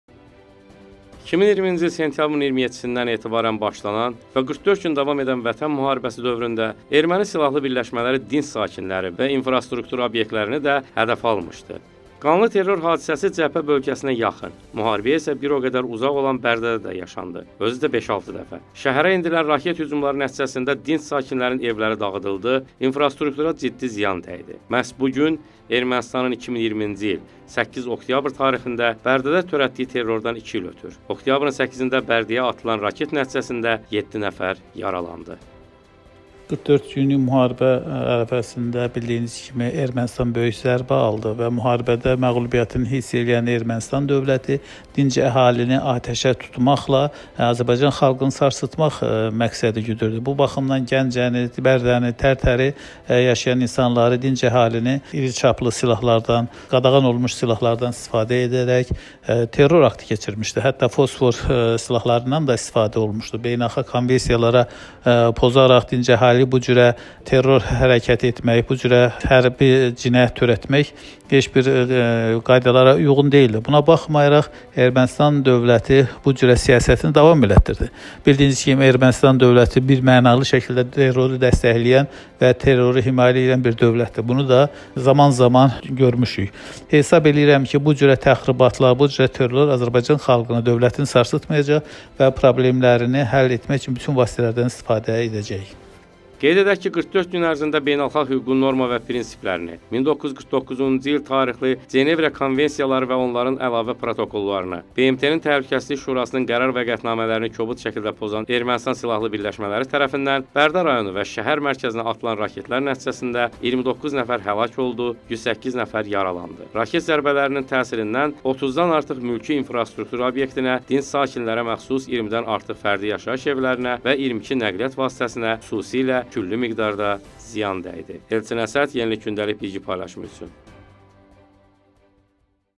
29 NƏFƏRİN HƏLAK OLDUĞU BƏRDƏ TERRORU
2020-ci il sentyabrın 27-dən etibarən başlanan və 44 gün davam edən Vətən müharibəsi dövründə erməni silahlı birləşmələri dinc sakinləri və infrastruktur obyektlərini də hədəfə almışdı. Qanlı terror hadisəsi cəbhə bölgəsinə yaxın, müharibəyə isə bir o qədər uzaq olan Bərdə də yaşandı, özü də 5 dəfə. Şəhərə endirilən raket hücumları nəticəsində dinc sakinlərin evləri dağıldı, infrastruktura ciddi ziyan dəydi. Məhz bu gün Ermənistanın 2020-ci il 8 oktyabr tarixində Bərdədə törətdiyi terrordan iki il ötür. Oktyabrın 8-də Bərdəyə atılan raket nəticəsində 7 nəfər yaralandı. Qeyd edək ki, 44 gün ərzində beynəlxalq hüququn norma və prinsiplərini, 1949-cu il tarixli Cenevrə konvensiyaları və onların əlavə protokollarını, BMT-nin Təhlükəsizlik Şurasının qərar və qətnamələrini kobud şəkildə pozan Ermənistan silahlı birləşmələri tərəfindən Bərdə rayonu və şəhər mərkəzinə atılan raketlər nəticəsində 29 nəfər həlak oldu, 108 nəfər yaralandı. Raket zərbələrinin təsirindən 30-dan artıq mülki infrastruktur obyektinə, dinc sakinlərə məxsus 20-dən artıq fərdi yaşayış evlərinə və 22 nəqliyyat vasitəsinə xüsusilə külli miqdarda ziyan dəydi.